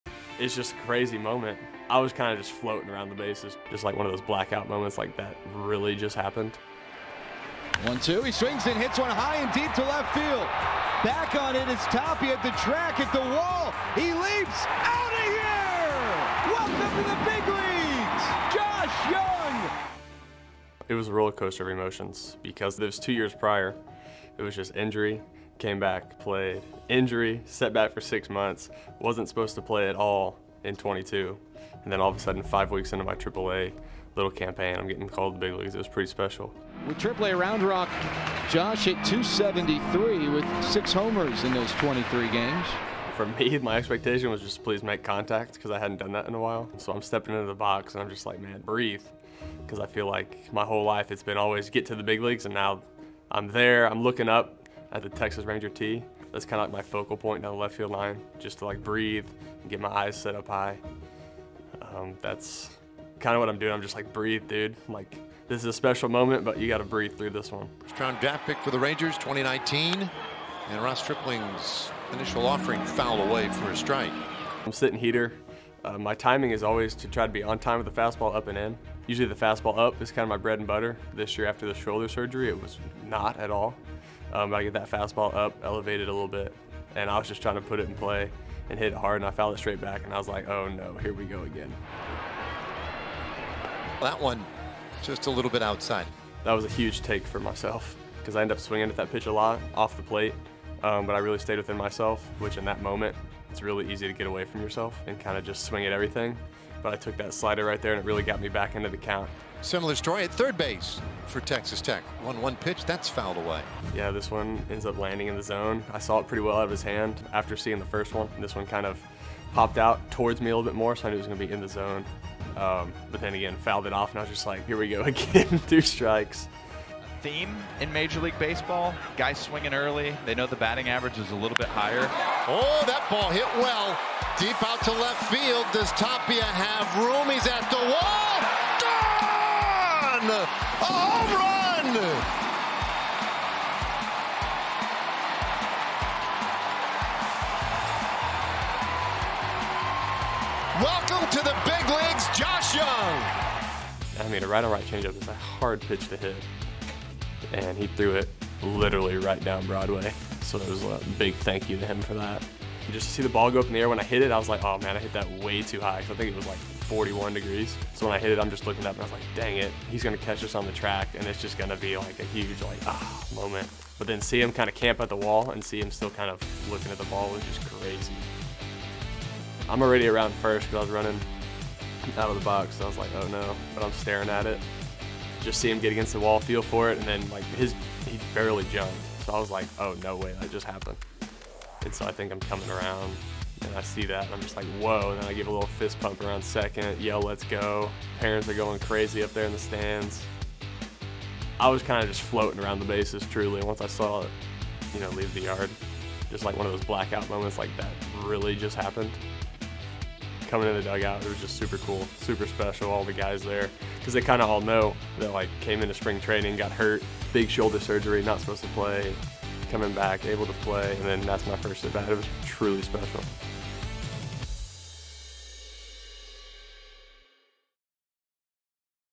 Rangers Rewind: Josh Jung Homers In First MLB At-Bat
Rangers Third Baseman Josh Jung breaks down his home run in his first ever at-bat in the big leagues on September 9, 2022 against the Blue Jays.

Re-live the moment with Jung in another episode of our Rangers Rewind series.